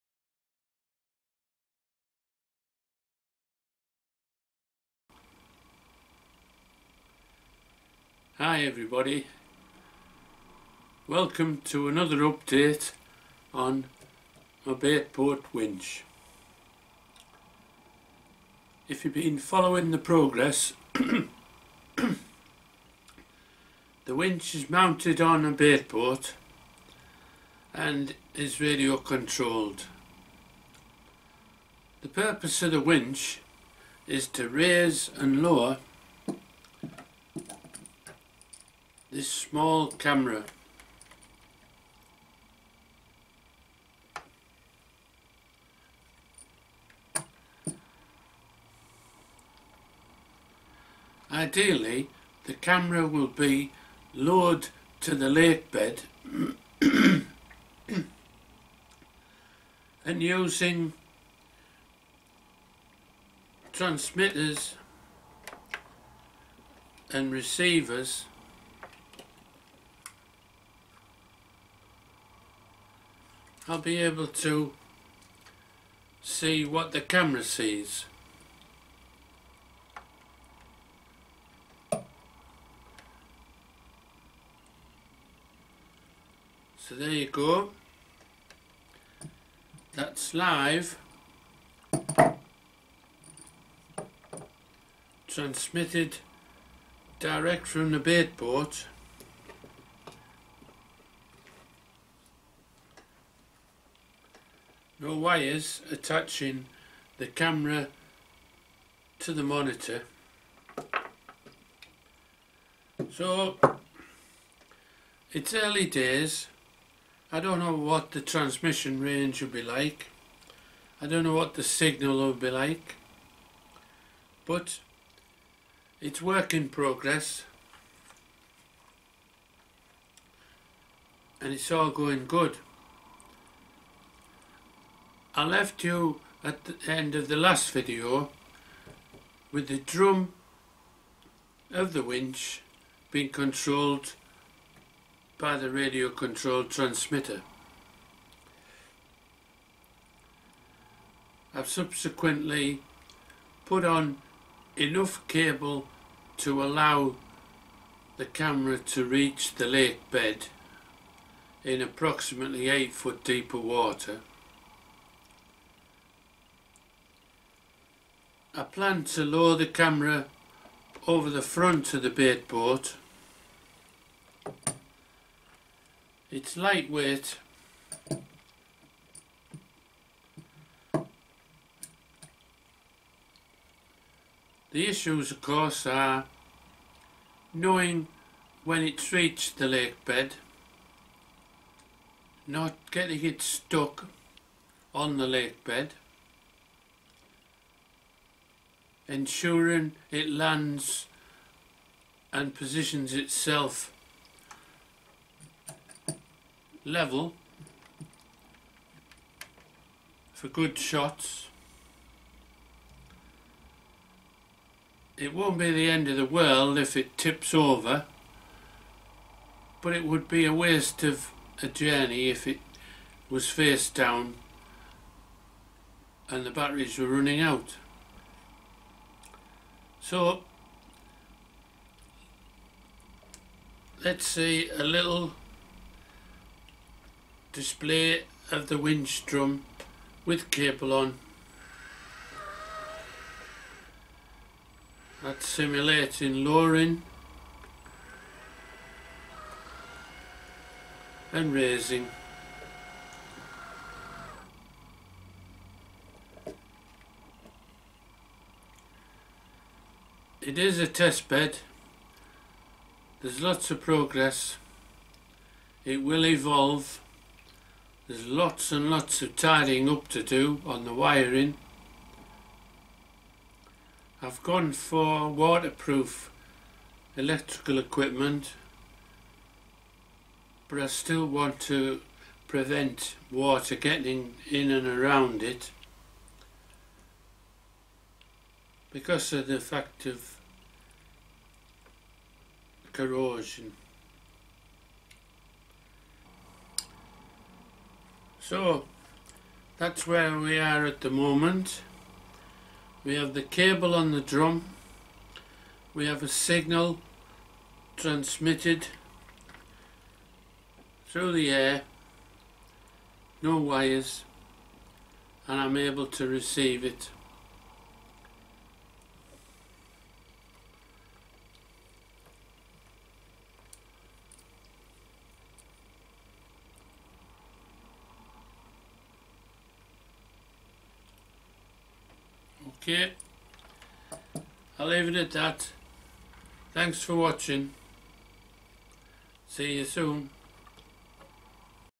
Baitboat winch with FPV Camera
progress report on Baitboat Winch with FPV Camera, for underwater photogrphy. In this report I show the waterproof camera, the wireless transmitter and receiver and the radio controlled winch drum .